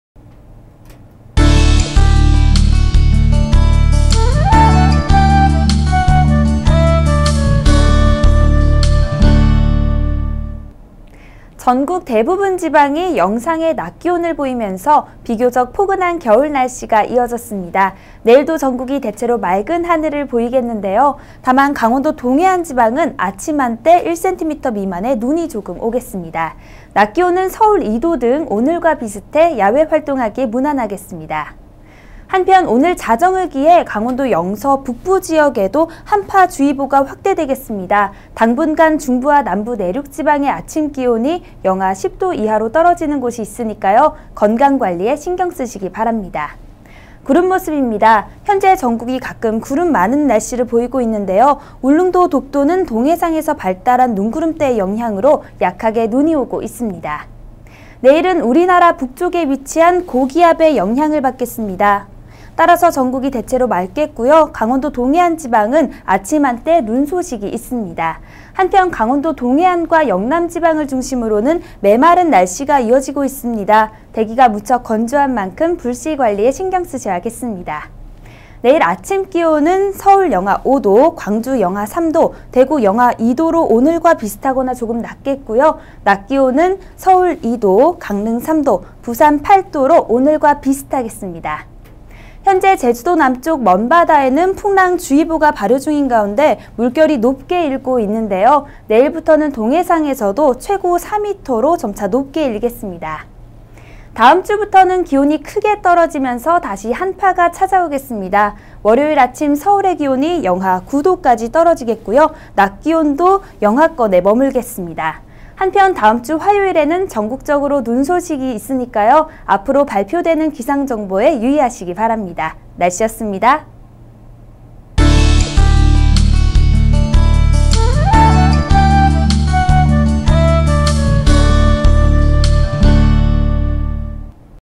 [날씨정보 2012년 1월 28일 17시] 내일(일) 전국 대체로 맑음
▶ 내일(일) 전국 대체로 맑음
- 강원동해안지방 아침 한때 눈 조금.
- 강원도영서북부*중북부산간지방 한파주의보(중부*남부내륙 아침기온 -10도↓)
- 강원동해안*영남*전남동부남해안 일부지방 건조주의보
- 다음주 한파 시작.